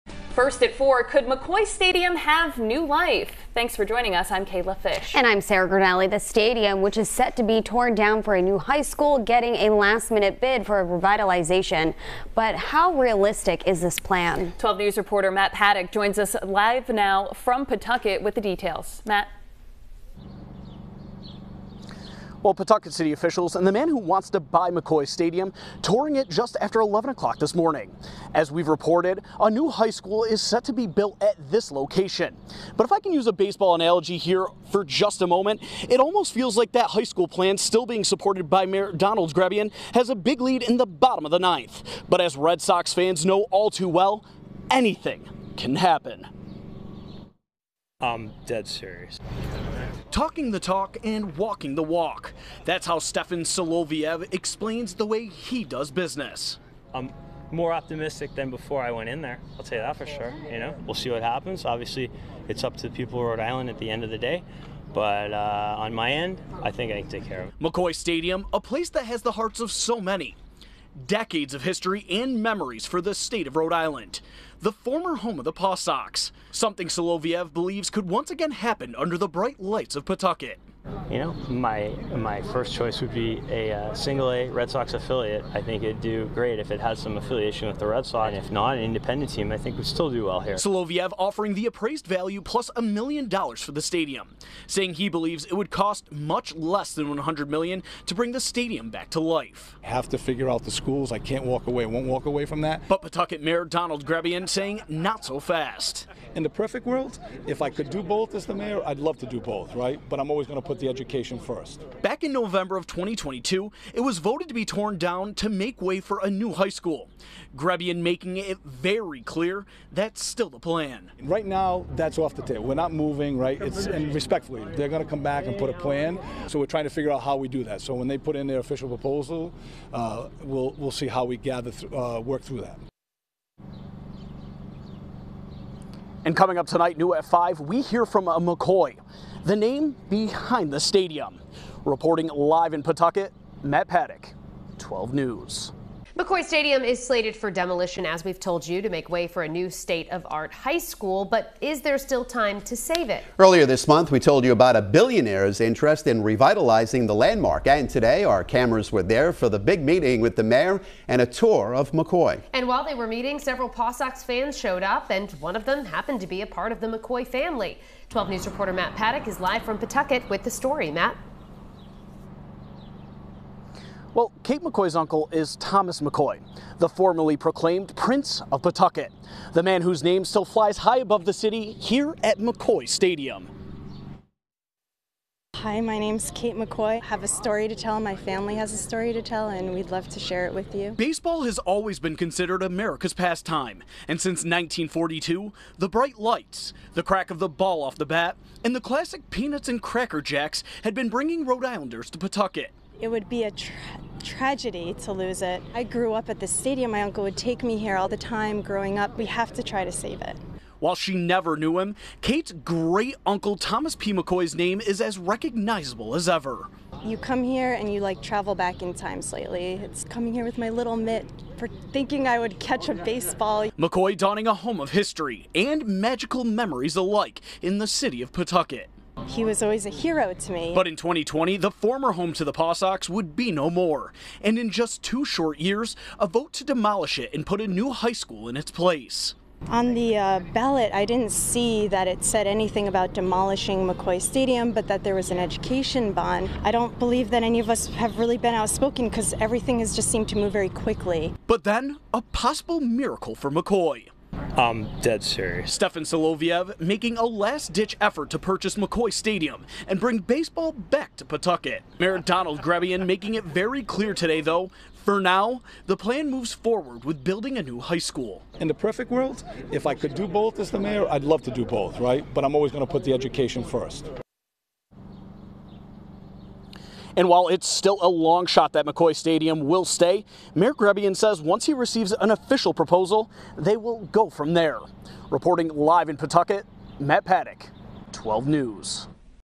Billionaire ‘dead serious’ about restoring McCoy to former glory
The billionaire looking to breathe new life into McCoy Stadium tells 12 News he’s optimistic about his last-minute bid to salvage the beloved Pawtucket ballpark.